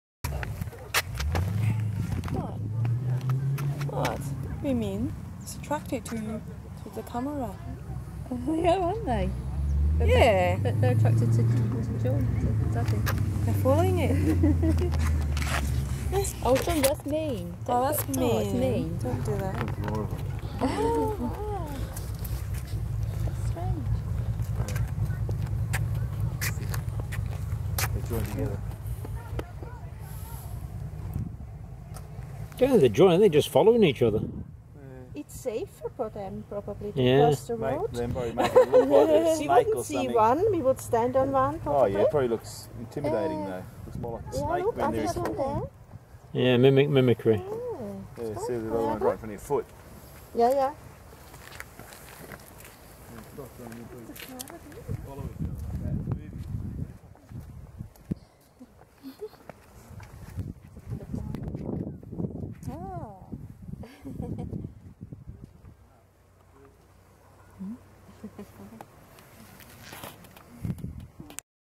Caterpillars in Western Australia
A chain of caterpillars on the way to the Pinacals in WA.